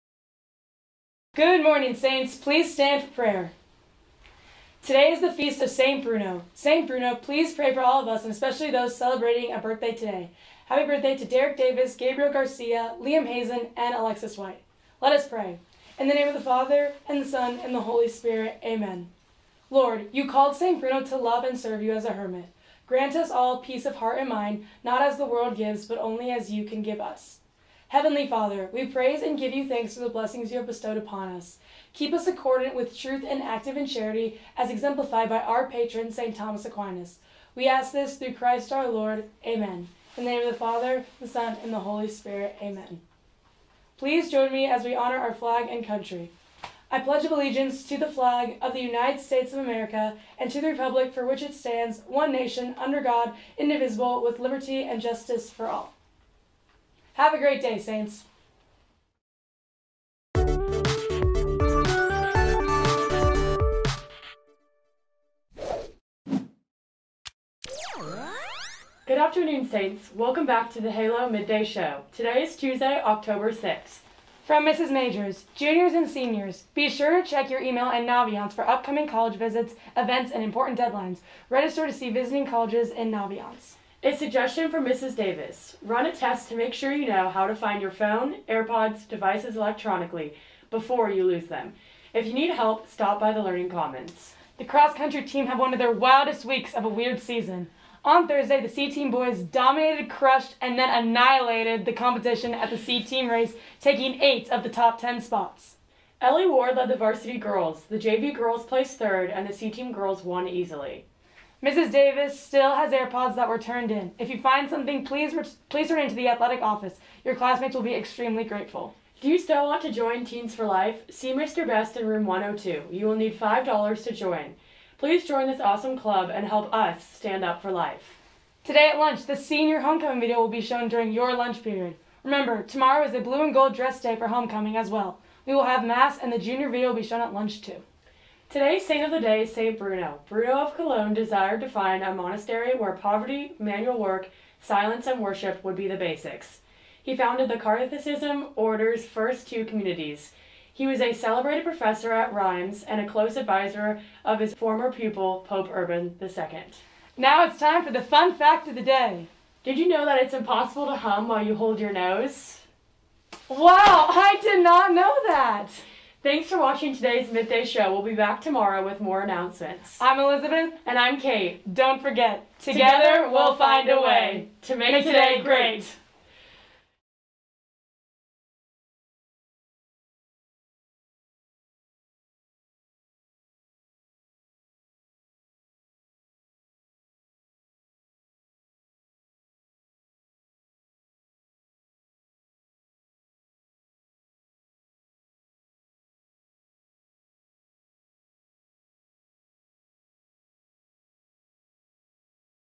The Halo Midday Show 10/6/20
Morning announcements and more for Tuesday, October 6th, 2020